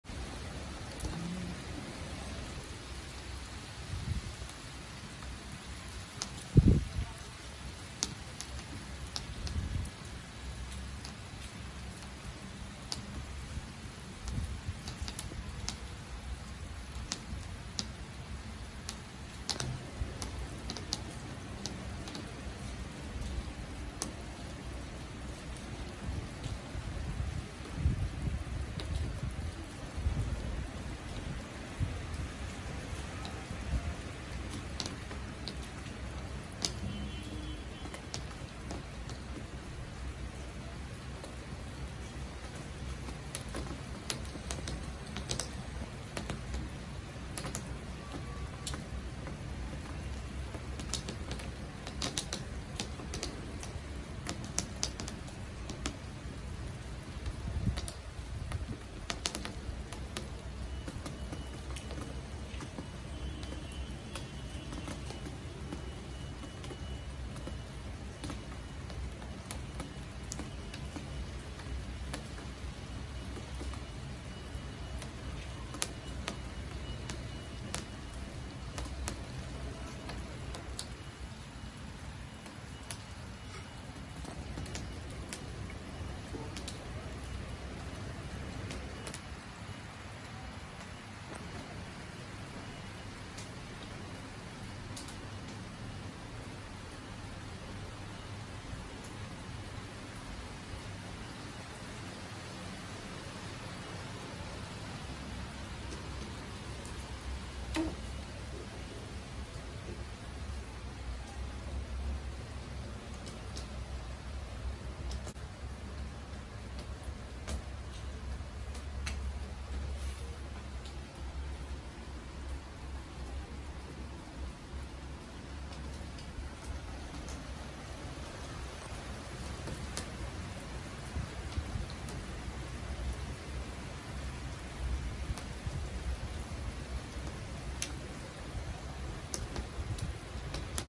Terasım Çiçeklerim Yağmur İstanbul
Yağmurlu bir İstanbul sabahı. Balkon, teras, çiçek ve doğal sesler. İyi seyirler. 🧿
kozalak şurubu:
romatizma ağrıları için yağ: